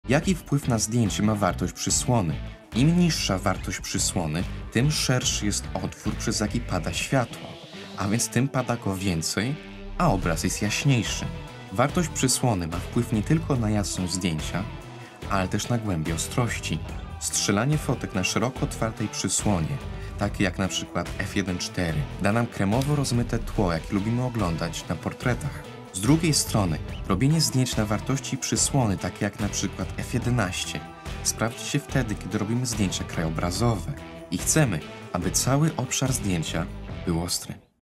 Jaki wpływ ma przysłona na jasność zdjęcia i głębię ostrości?
Przysłona to bardzo ważny element trójkąta ekspozycji. Przydaje się kiedy podejmujemy decyzje jakiego rodzaju zdjęcie chcemy zrobić: Portret lub krajobraz. Kiedy chcemy wpuścić dużo światła lub je ograniczyć.